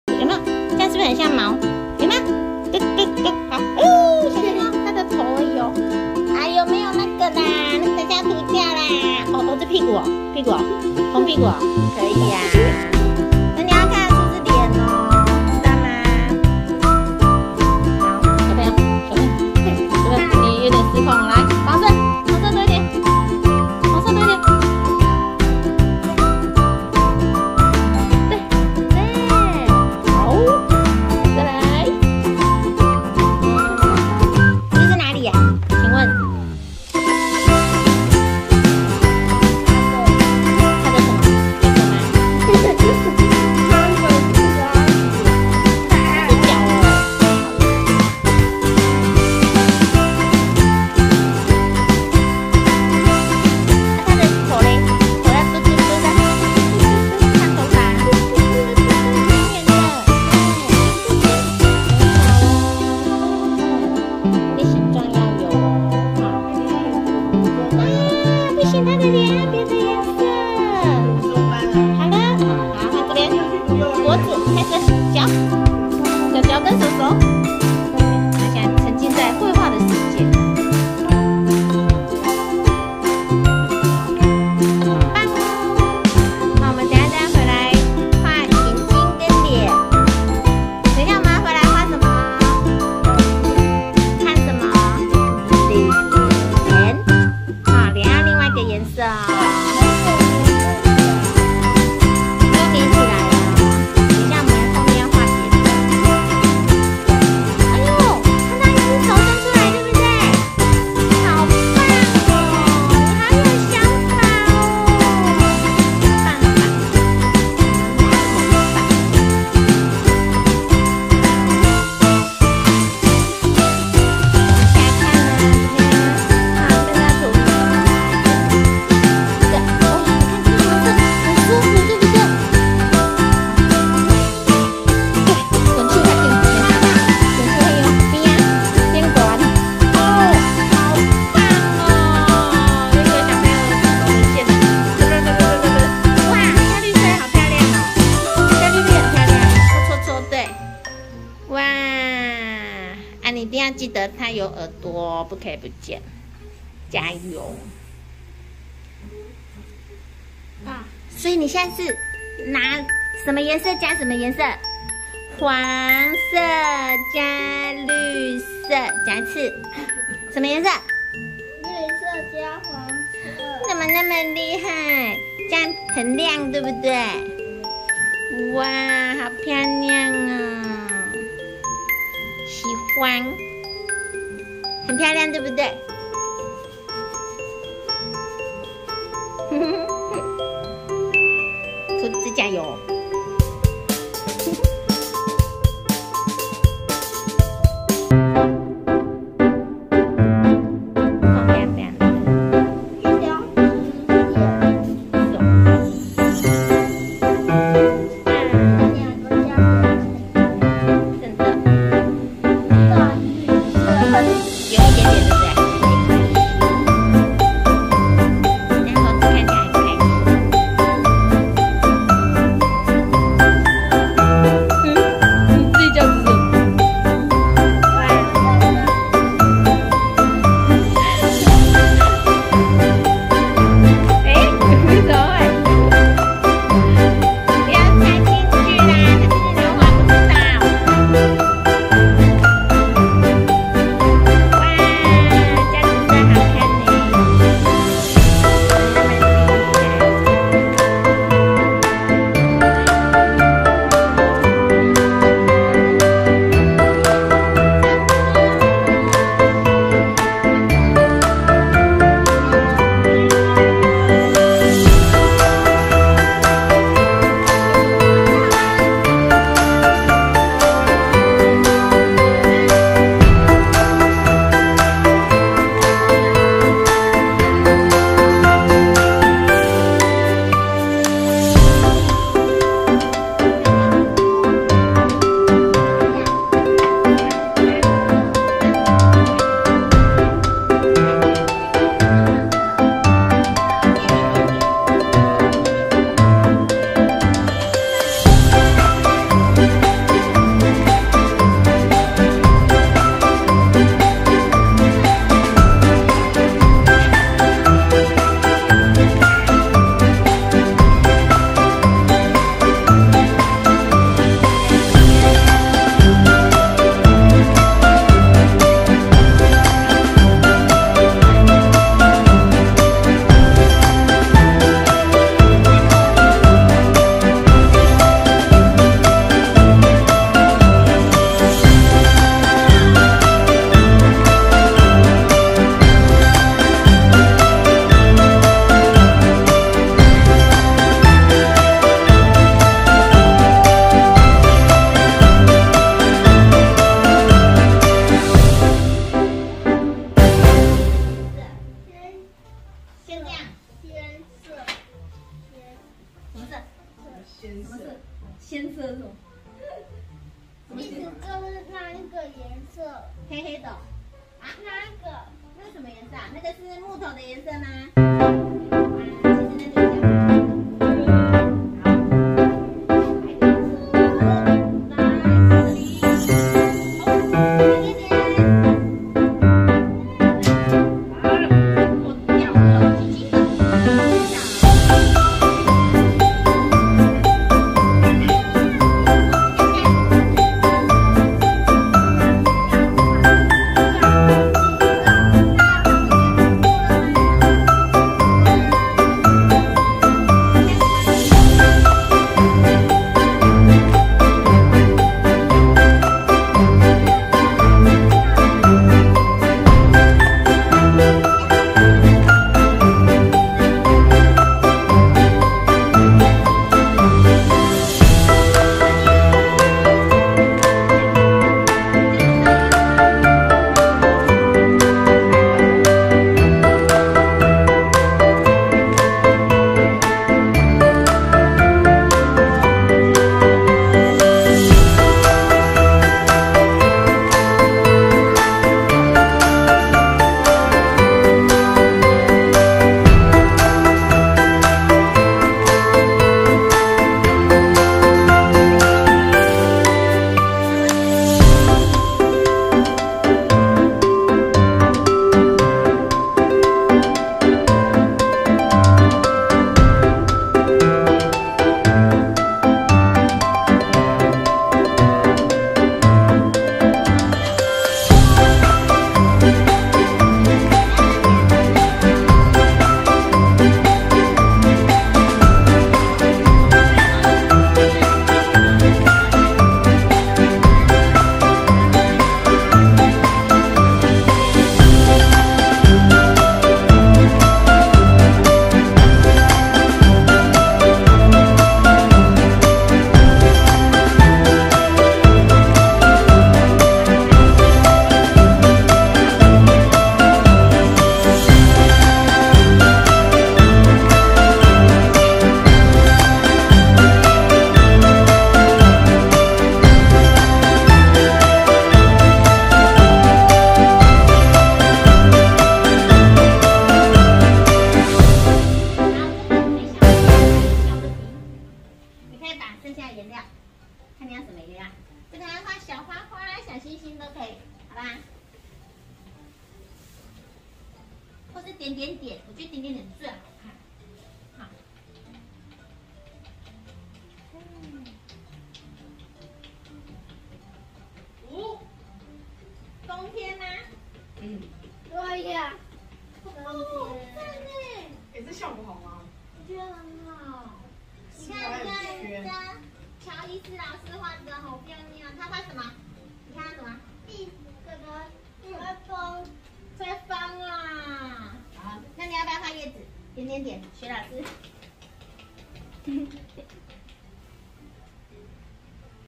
靜花園的油畫課，五歲男童篇
個別化適性引導式教學，油畫的視聽觸動五感刺激，每節課1-1.5小時(視孩童專注力)